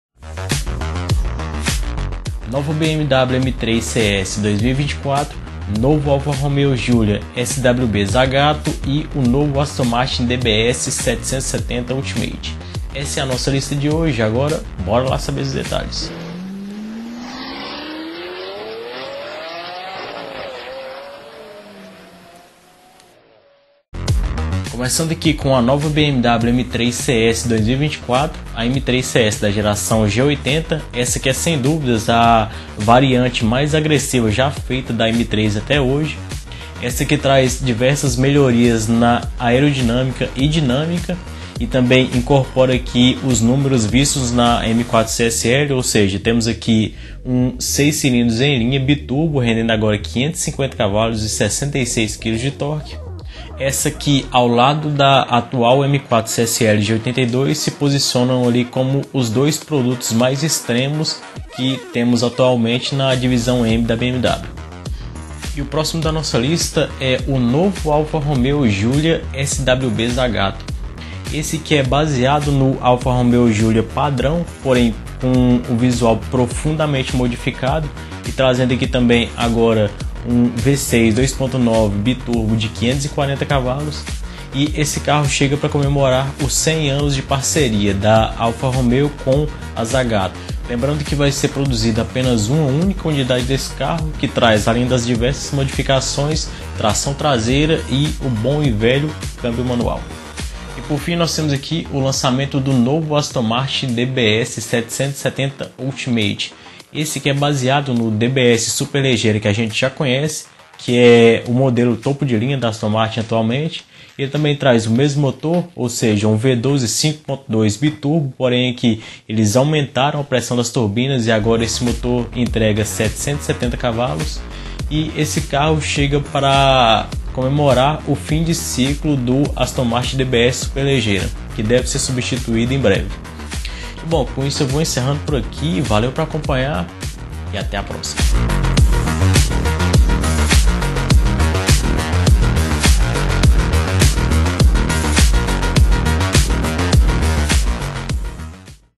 BMW M3 CS 2024, Alfa Romeo Giulia SWB Zagato e mais! │ Car news
A variação mais radical do BMW M3! Hoje trago mais detalhes do novo BMW M3 CS 2024 g80, novo Alfa Romeo Giulia SWB Zagato e o novo Aston Martin DBS 770 Ultimate que possivelmente será a despedida da atual geração do Aston Martin DBS.